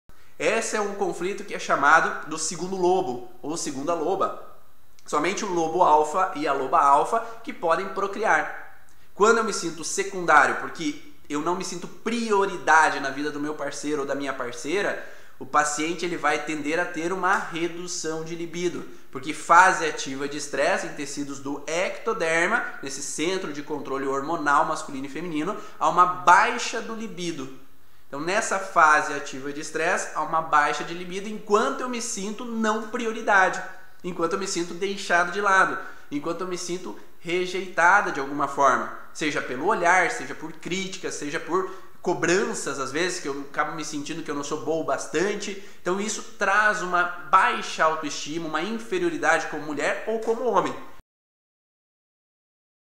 Terapeuta, você conhece o conflito do segundo lobo? | Ivan Bonaldo
Somente o lobo ou a loba alfa conseguem procriar. Como isso pode colaborar para compreender relações e ajudar nosso paciente? Aprenda sobre esse conflito para obter o resultado eficiente com o tratamento, em relação à libido e as relações íntimas.